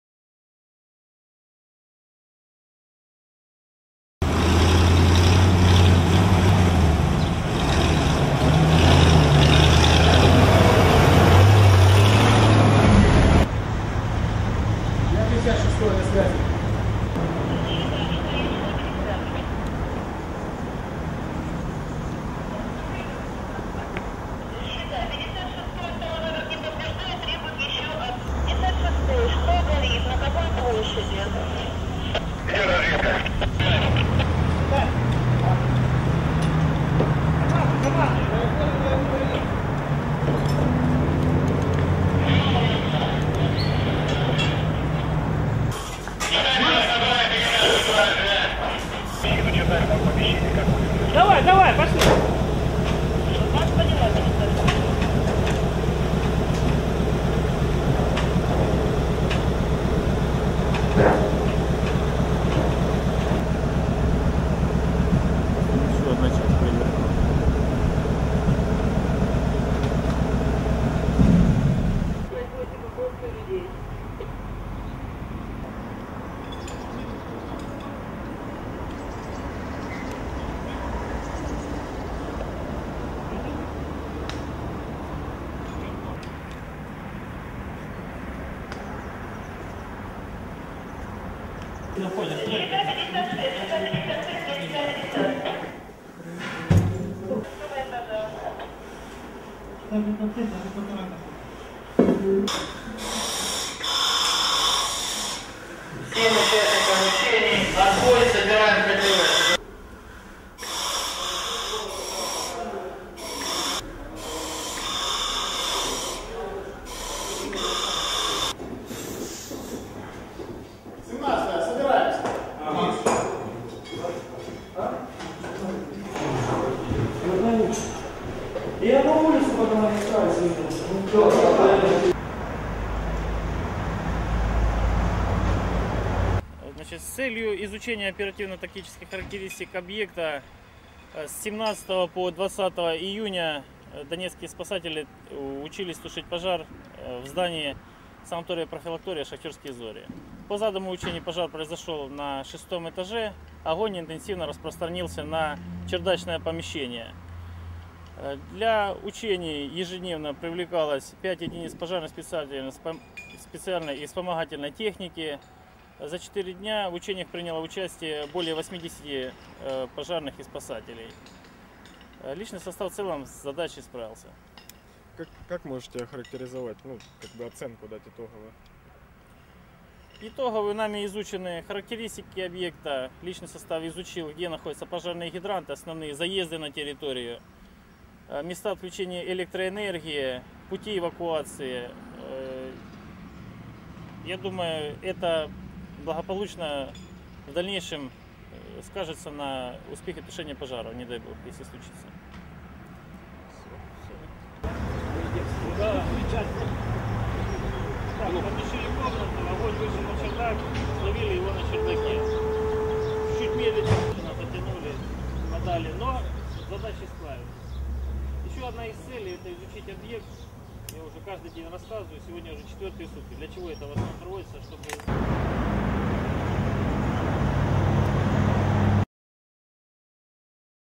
Донецьк: проведено тактико-спеціальні навчання
20 червня у Донецьку проведені тактико-спеціальні навчання з організації  гасіння умовної пожежі, яка виникла у санаторії-профілакторії «Шахтарські зорі». За легендою навчань, через коротке замикання електричної мережі загоряється постільна білизна в кімнаті на 6-му поверсі, на якому знаходяться близько 52 людини. При спробі евакуюватися люди не змогли відчинити двері на сходи та зібралися біля вікна в кінці коридору.

Для виконання навчального завдання на місце пожежі прибули 2 автоцистерни та автодрабина. В ході розвідки було встановлено, що з вікна шостого поверху центрального корпусу виривається відкрите полум'я, від нього загорілася покрівля. Проводиться евакуація людей силами персоналу об'єкту.

Як відмітив оперативний черговий оперативно-диспетчерського відділу оперативно-координаційного центру ГУ ДСНС України у Донецькій області майор служби цивільного захисту Віталій Зарубін, який керував навчаннями: «Особовий склад, в цілому, з поставленим завданням впорався добре». Під час навчань були відпрацьовані способи евакуації людей з будівлі, рятування людей по автодрабині, перевірена оперативна готовність сил та засобів підрозділів до дій за призначенням.